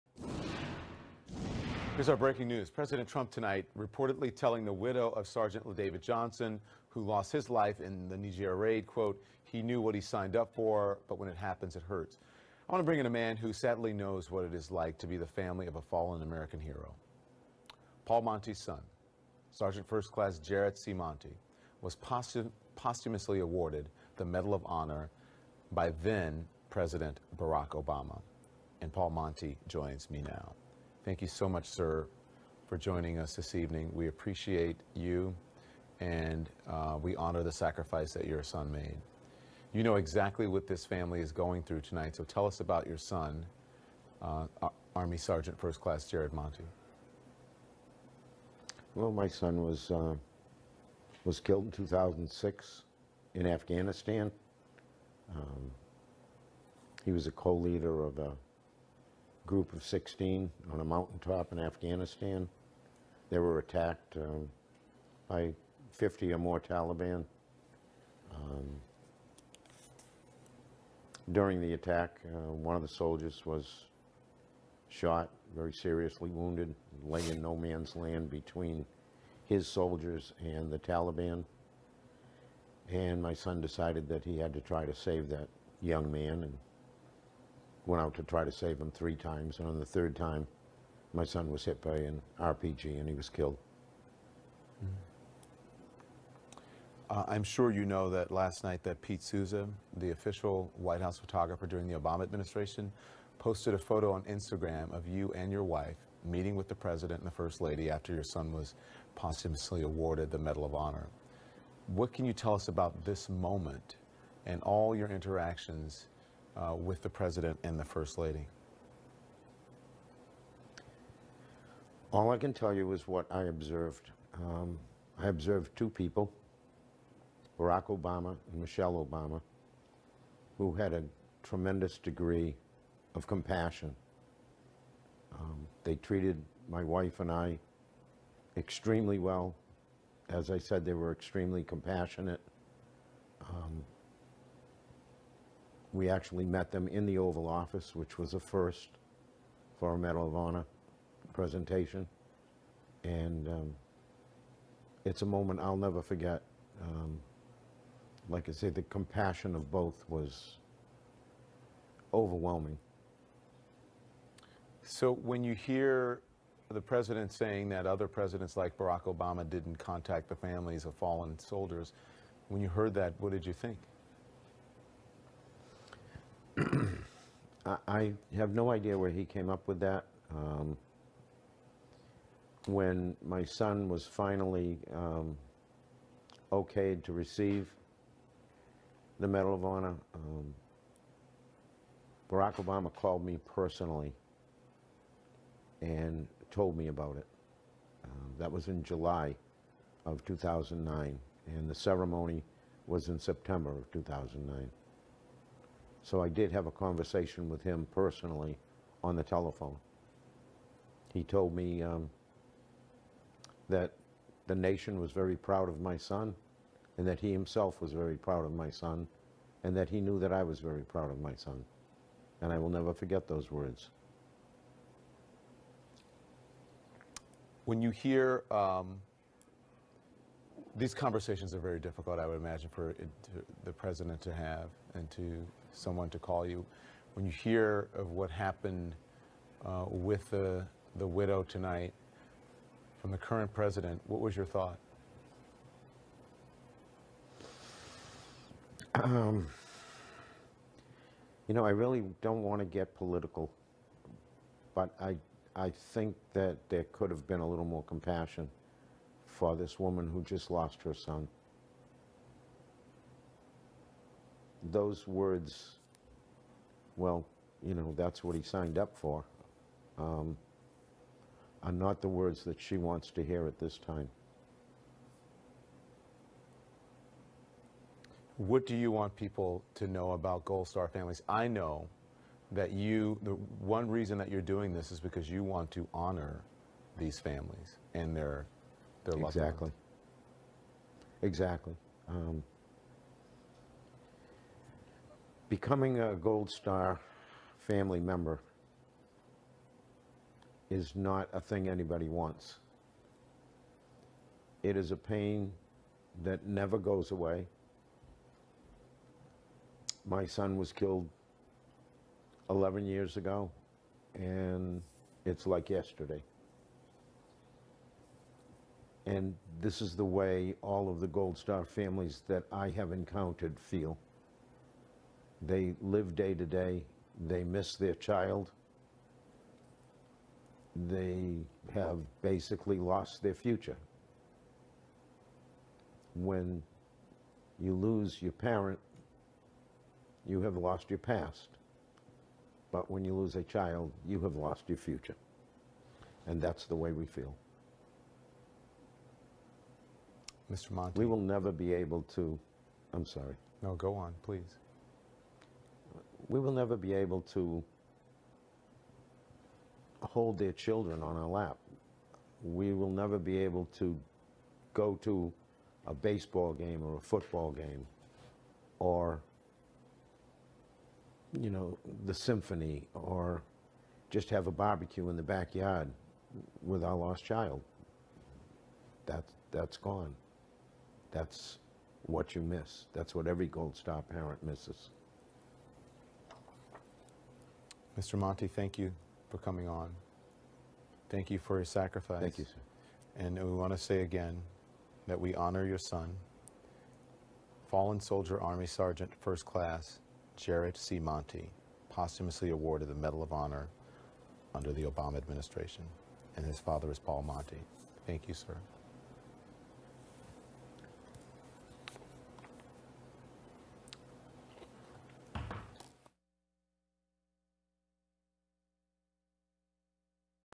Gold Star father speaks out on Trump remarks " he knew what he signed up for "
Gold Star father speaks out on Trump remarks to a pregnant widow of U.S. special forces soldier , one of four killed in Niger . CNN's Don Lemon spoke to Paul Monti, the father of fallen soldier Jared Monti, who recounted visiting the White House after his son's death and the phone call he received from Obama when his son was awarded the Medal of Honor

Donald Trump called the widow of Sgt. La David Johnson on Tuesday afternoon
Myeshia Johnson was on her way to Miami airport where her husband's remains were arriving when she took the President's call
Rep. Frederica Wilson said she was sitting beside the grieving widow when Trump said the slain solider 'knew what he signed up for'
Heartbreaking footage later showed Johnson, who is pregnant with the couple's third child, sobbing over her husband's flag-draped coffin
Trump called on Tuesday the families of the four US troops who were killed in Niger nearly two weeks ago